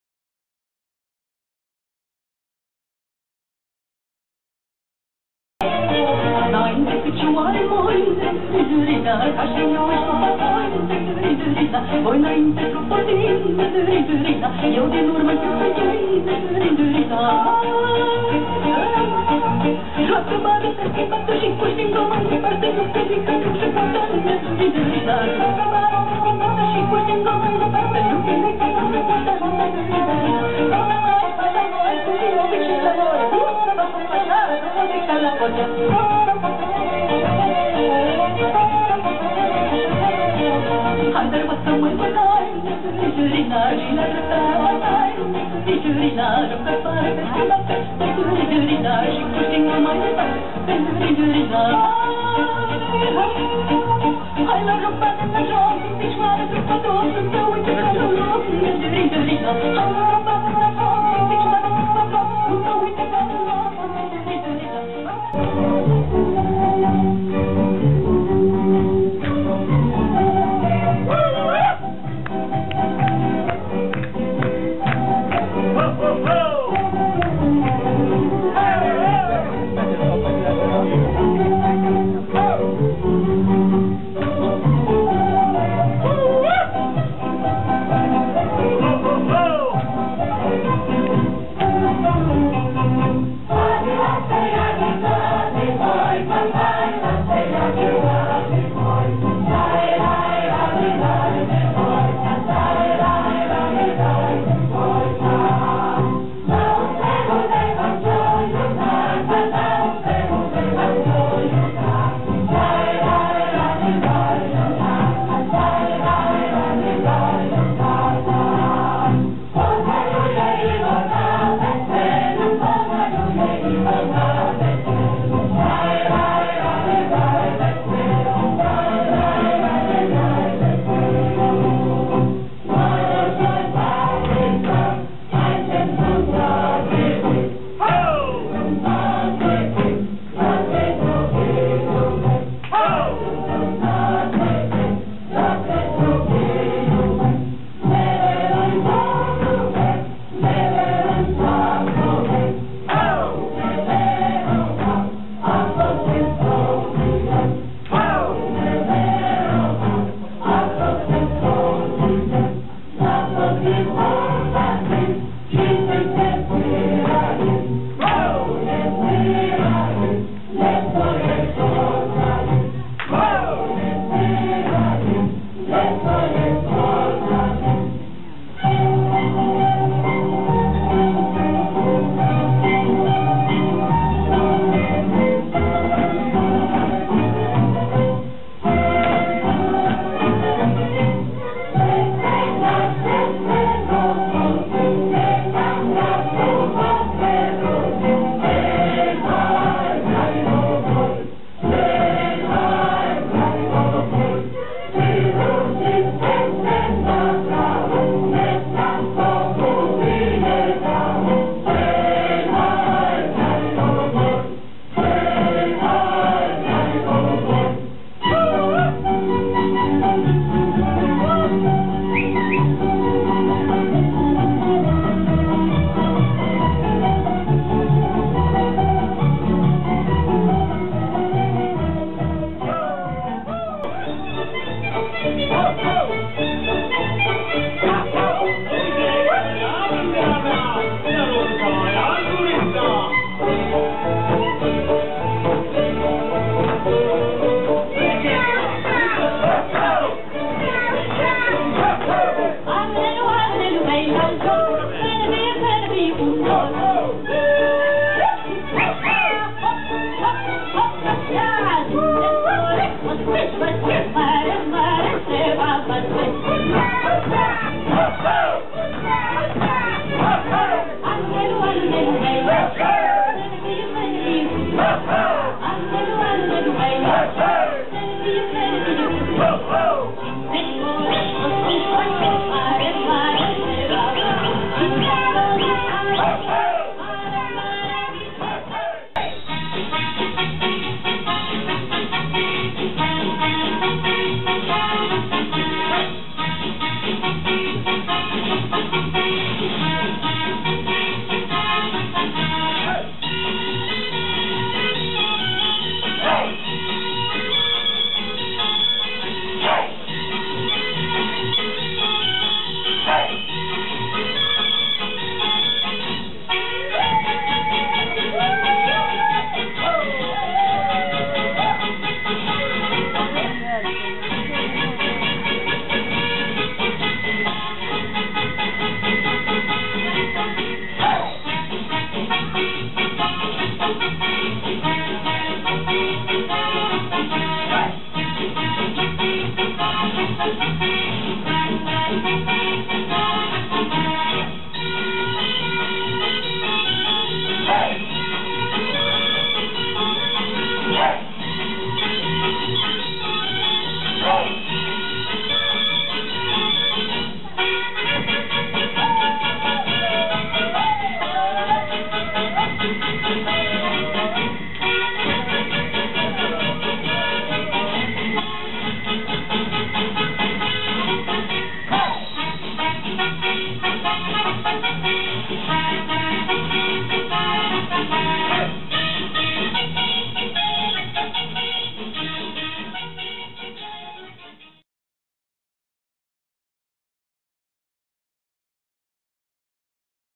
DANZE BALCANICHE.
FORMELLO 3 APRILE 2011 "Ballando Ballando". Spettacolo di beneficenza  a favore della Croce Rossa. Ensemble di danze balcaniche. Balla   Il Cerchio dell'Amicizia onlus